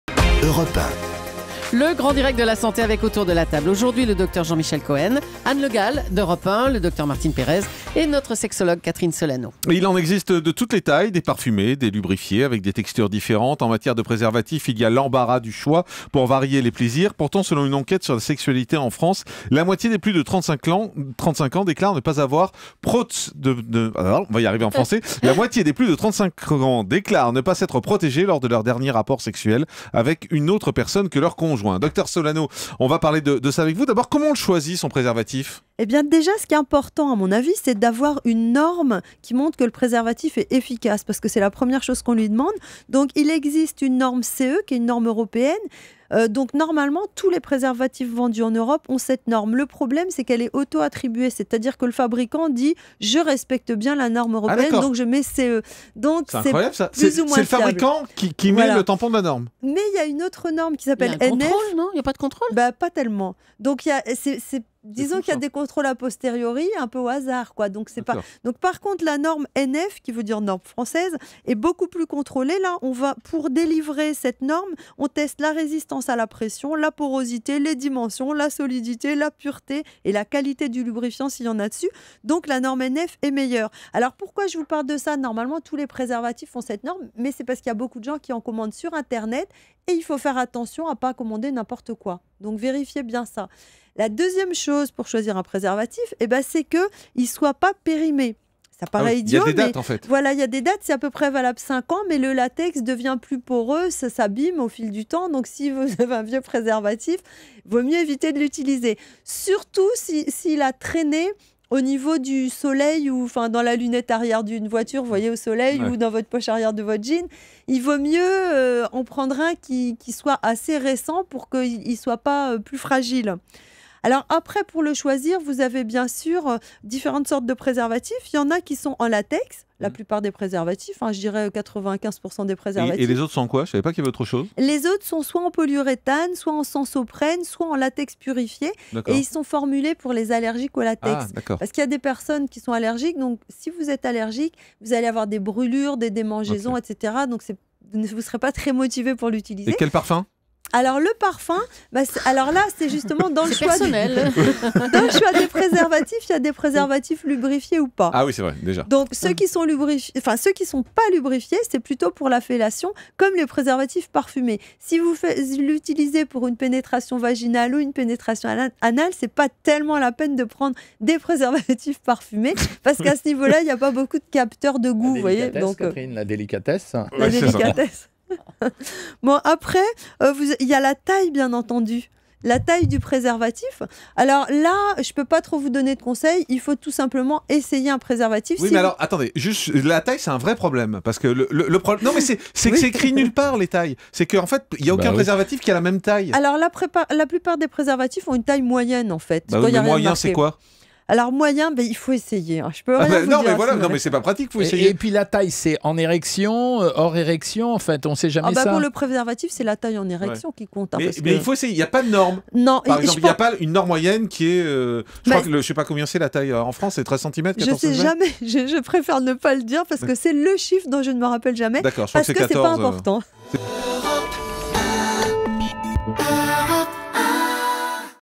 Le préservatif
Colorés, parfumés, lubrifiés, en matière de préservatifs, nous avons l'embarras du choix. Cependant cela n'est manifestement toujours pas un réflexe pour tout le monde. Catherine Solano, sexologue, nous refait un point.